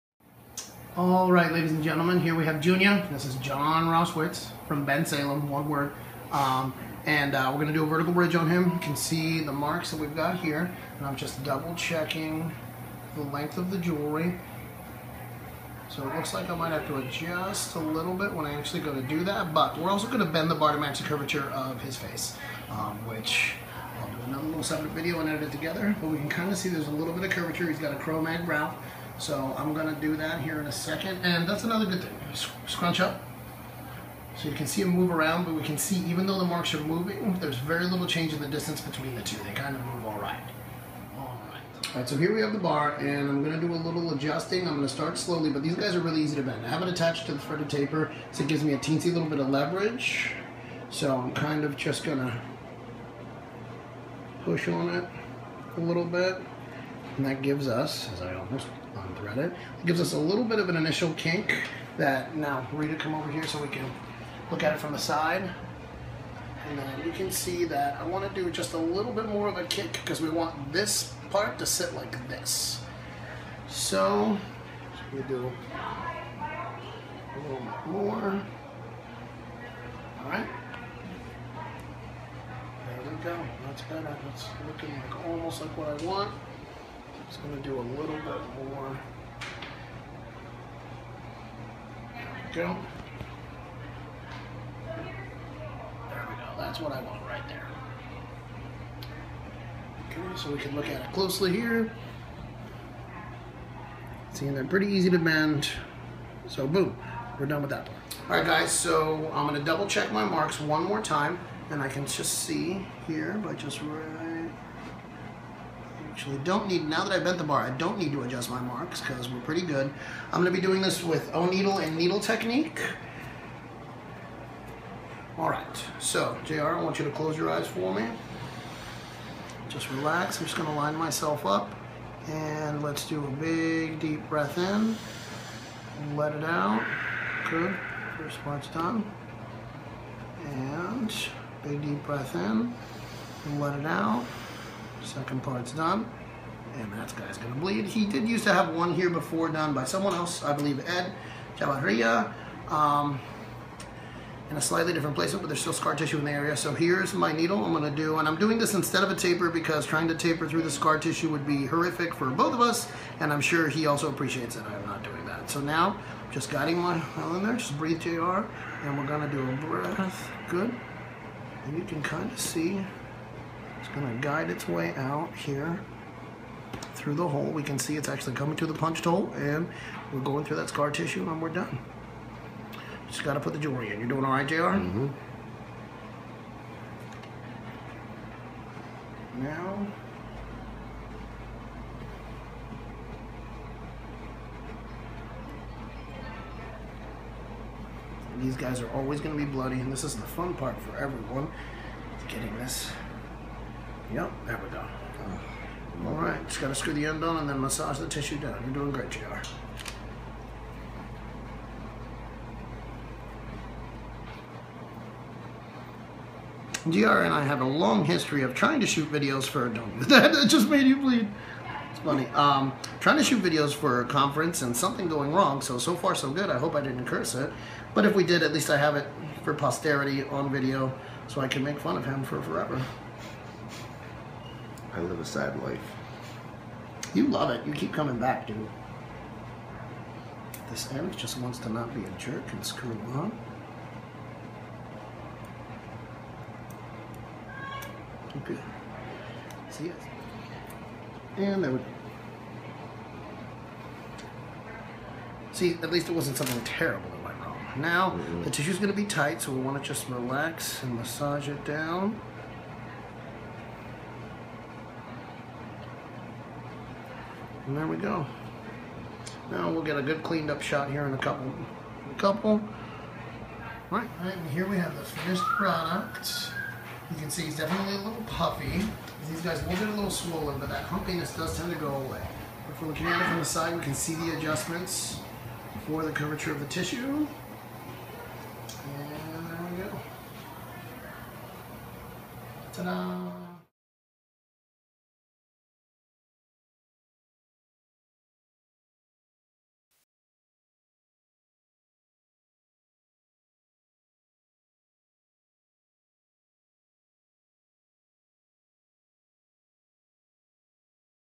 Vertical bridge surface bar procedure by Luis Garcia (me)
Vertical bridge surface bar procedure done via O needle and blade needle technique, also shows adjusting the bar to match the curvature of the area.